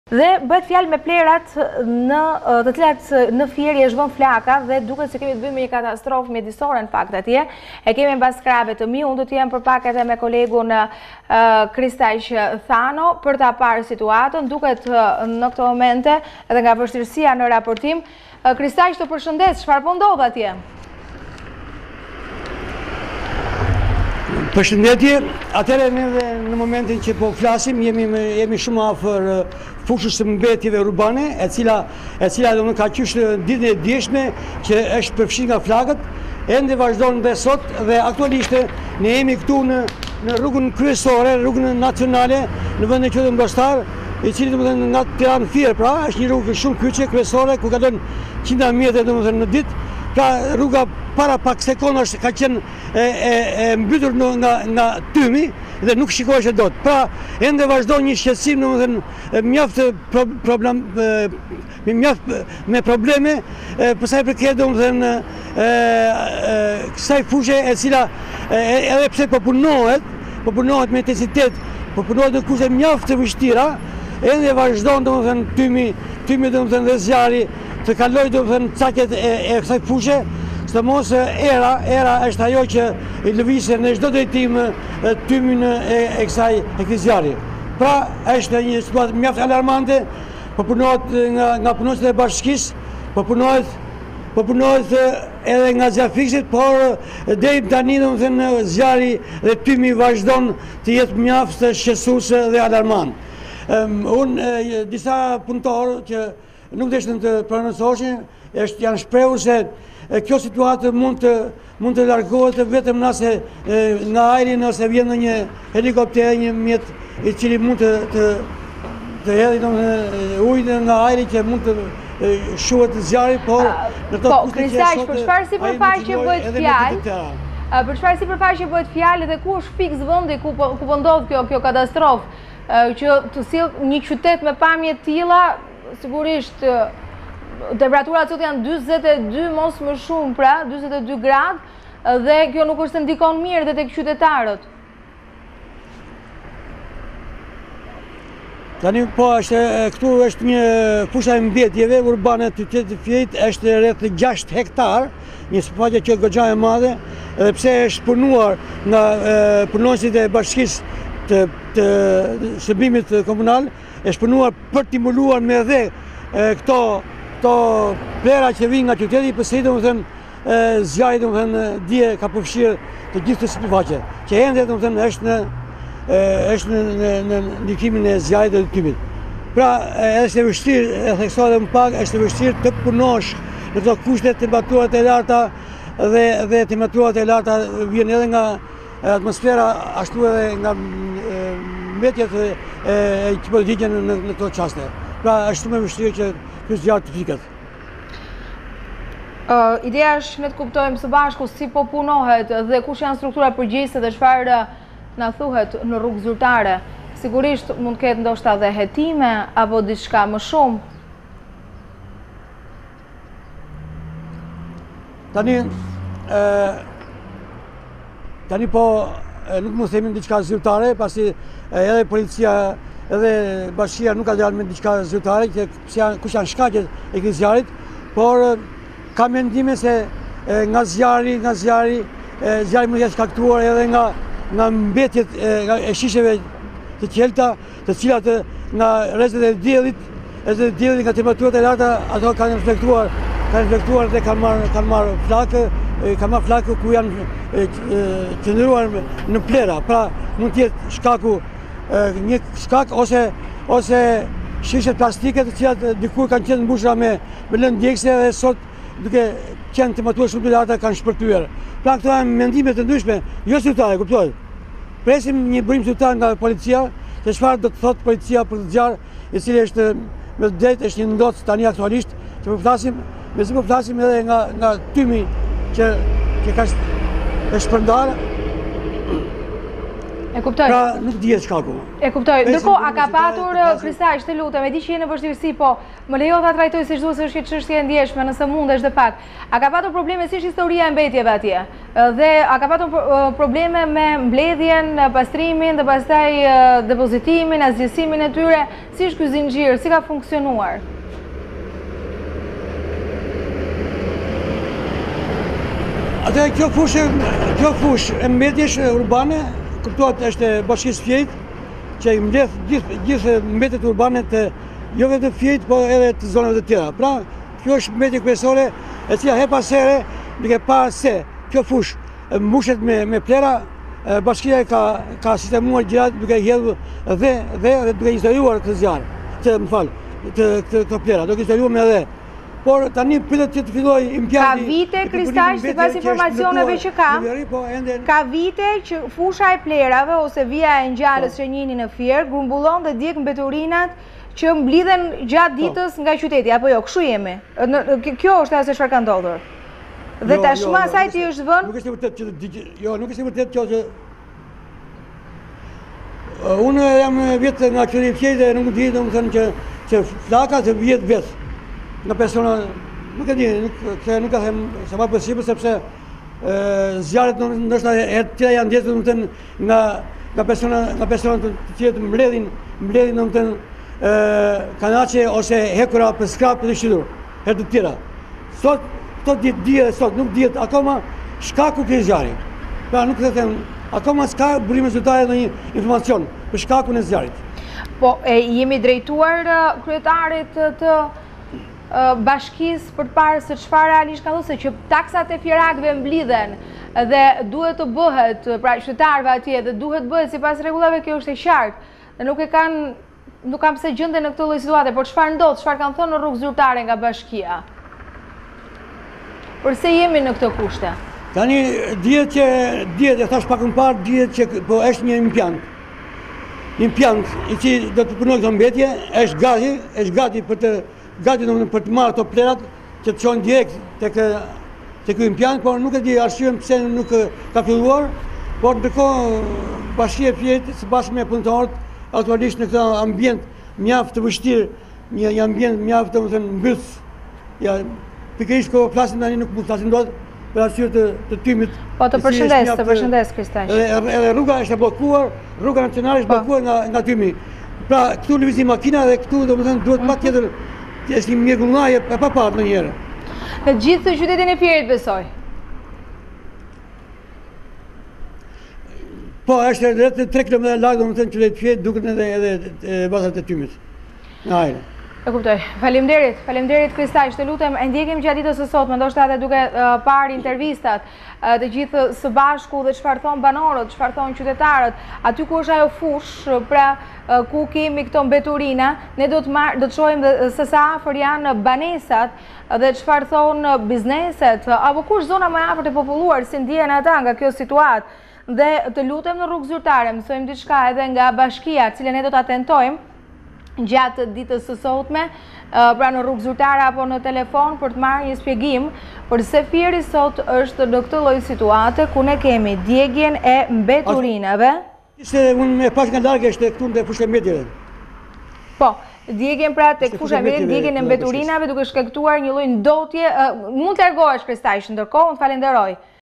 Report TV -Emergjencë mjedisore në Fier, tymi mbulon edhe qytetin, plehrat në flakë në landfill!
Qyteti i Fierit është mbuluar mëngjesin e sotëm nga një mjegull tymi nga zjarri i fushës së mbetjeve urbane e cila mori flakë ditën e djeshme. Një re e dendur tymi ka mbuluar qytetin si pasojë e erës. Banorët ankohen se prania e tymit ndjehet edhe në frymëmarrje.

Janë disa vatra zjarri në fushën e mbetjeve urbane në Fier e cila shtrihet në një hapësirë prej 6 hektarësh. Edhe pse ndodhet rreth 3-4 km larg qendrës së qytetit të Fierit ndikimi që ka era ka bërë që re të dendura tymi të 'pushtojnë' qytetin.